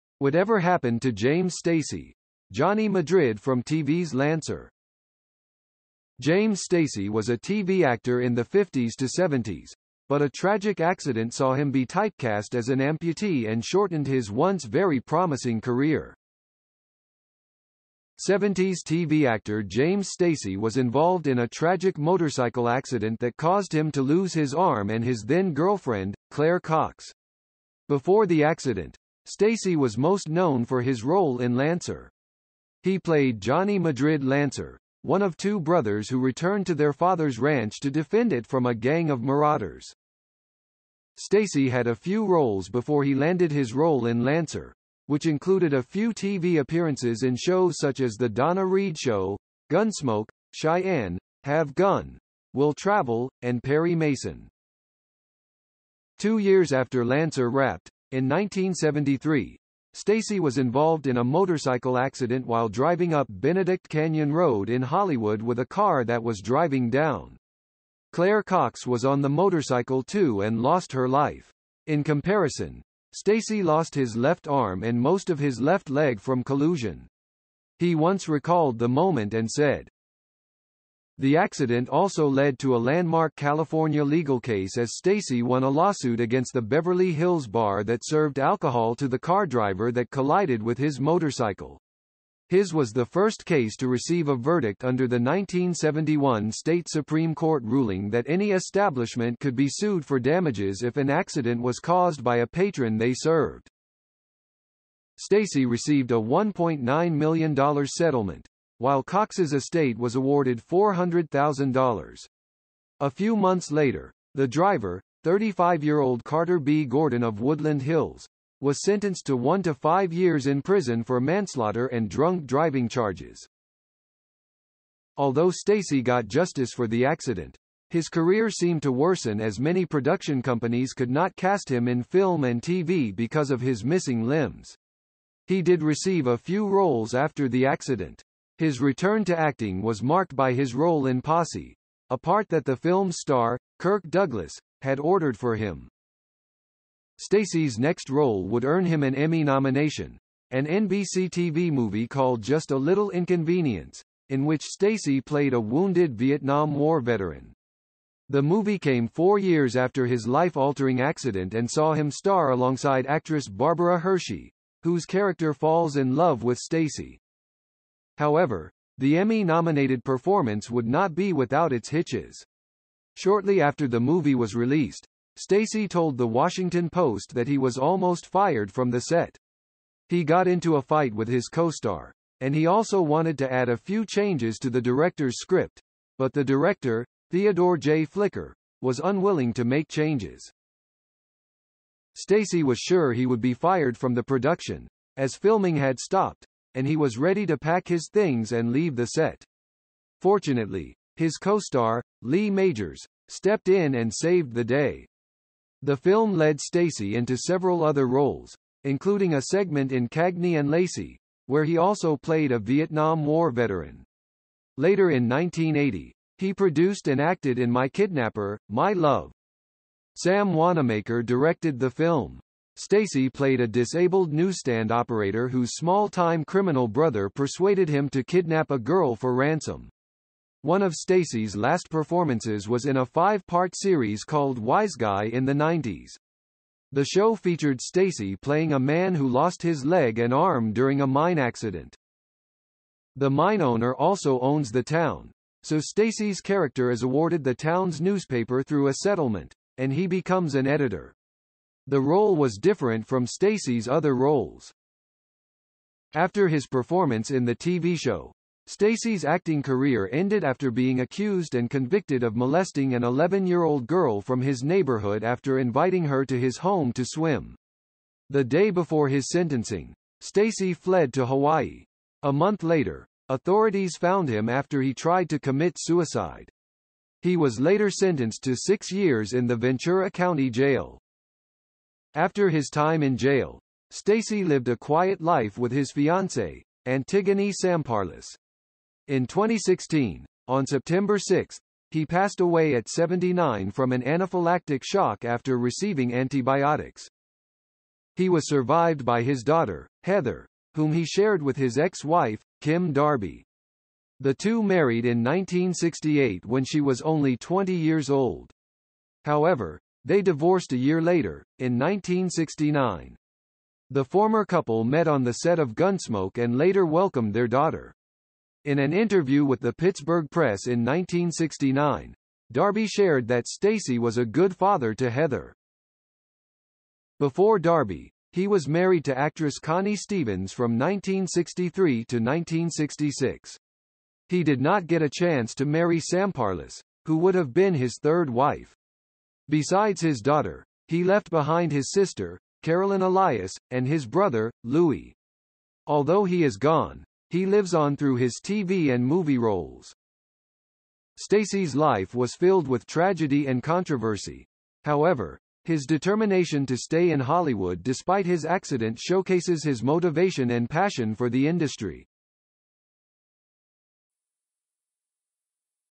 Whatever Happened to James Stacy - Johnny Madrid from TV's Lancer
James Stacy, better known by his stage name James Stacy, is an American film and television actor. He is perhaps best known for starring in the late 1960s western television series Lancer. James Stacy was involved in a tragic motorcycle accident that resulted in the loss of his arm and his then-girlfriend, Claire Cox.